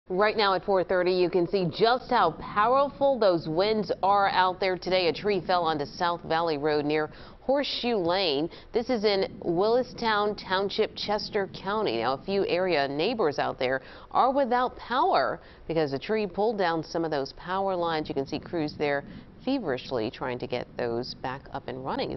Trees down in Willistown Township
A few area neighbors are without power because the tree pulled down some power lines.